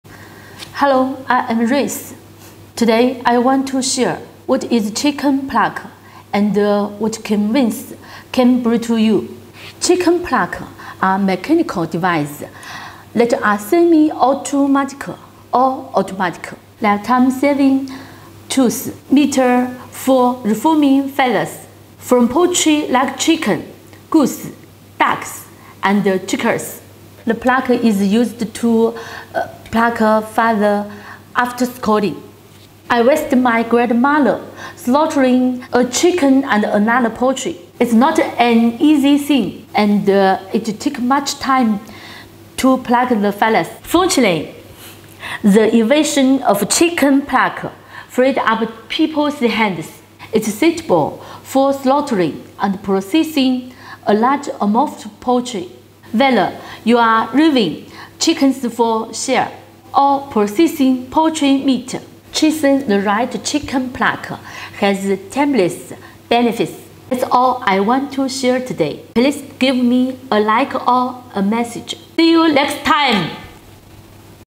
What's the chicken plucker?
A brief introduction of chicken pluckers from Rais.